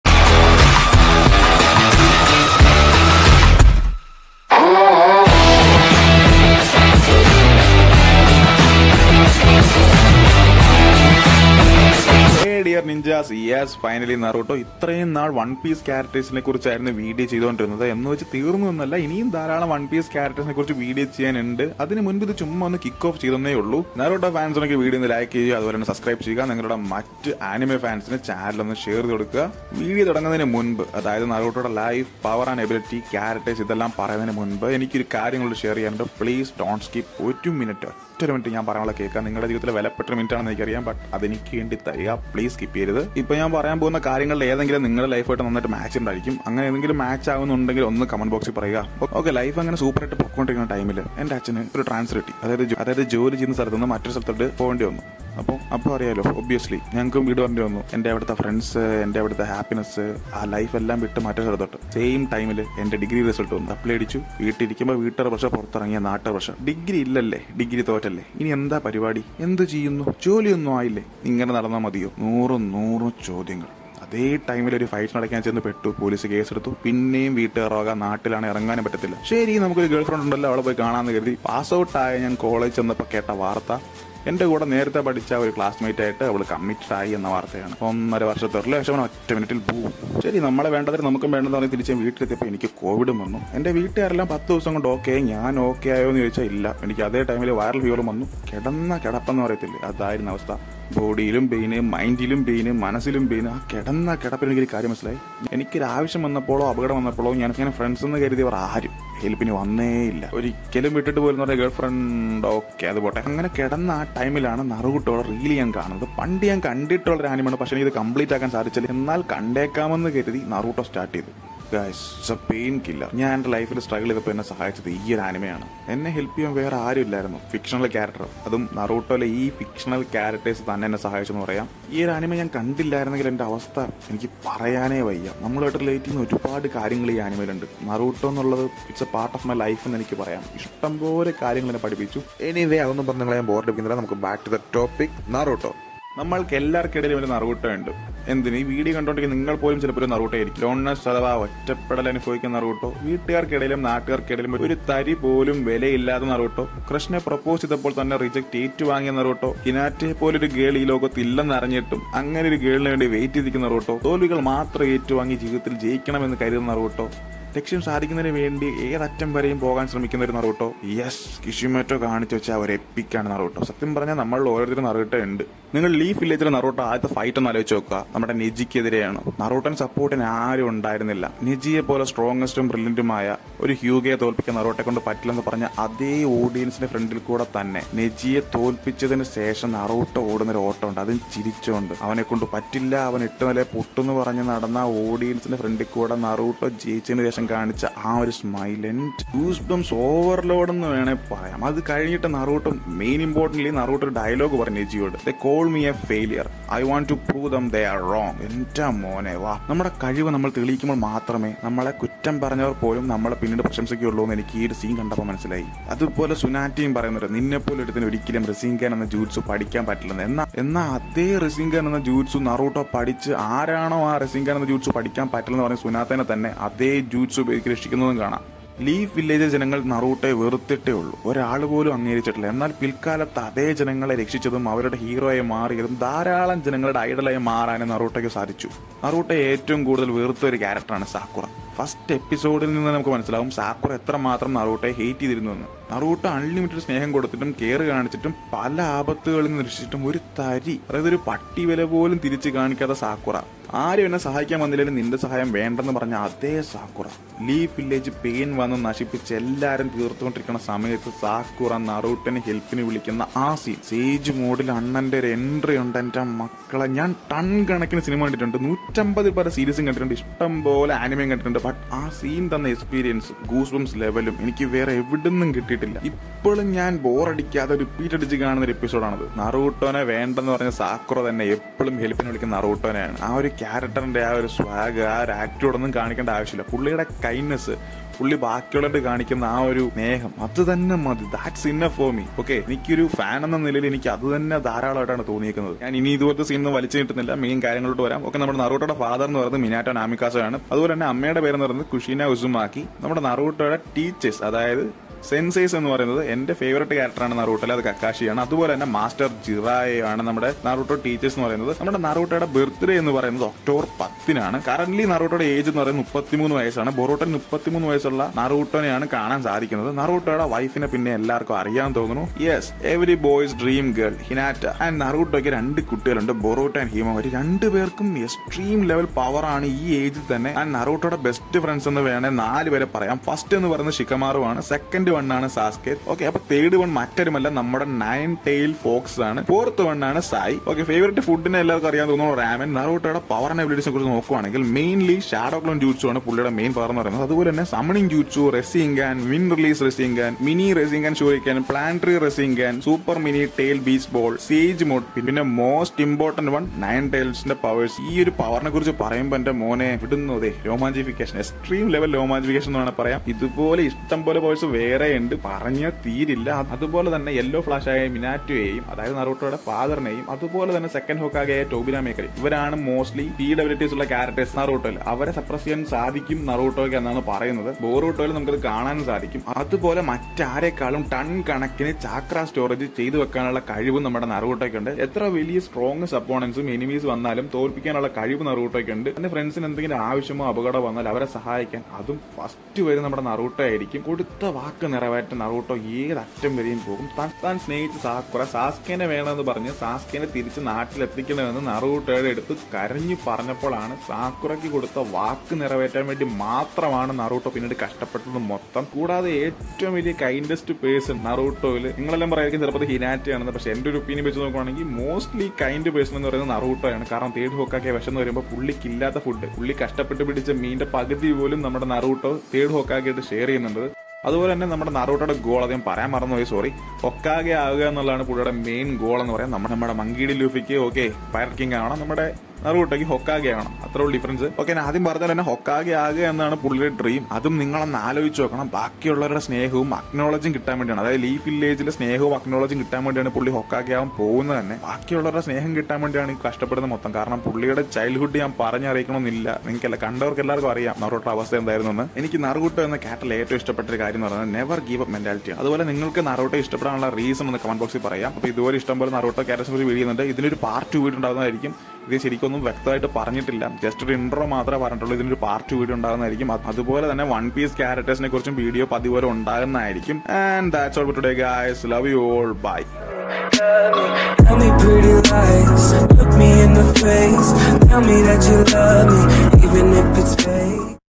Characteristics Of Naruto Uzumaki | Malayalam | Review And Explanation | COPY D VIPER
This Video is only about Naruto Uzumaki characteristics
tnx for your tym